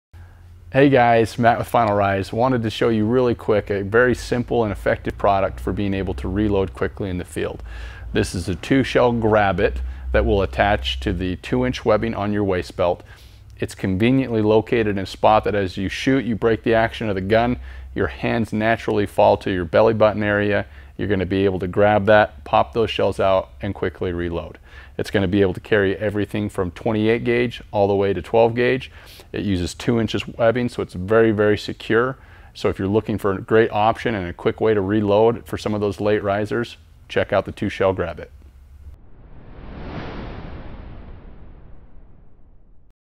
FINAL RISE 2 SHELL HOLDER OVERVIEW
ALWAYS READY

Designed to wrap around and sit securely on the 2" webbing of your waist belt, the FINAL RSIE Shell Holder is incredibly convenient for quick reloads. Whether it's a staggering covey flush, buckshot for coyotes and javelina or a quick-grab for the doubles at the sporting clays range, everyone can use one in their kit.

Product Details:
- Fits 12ga - 28ga shells. Will not hold .410 bore
- 2" Tall webbing to keep shells secure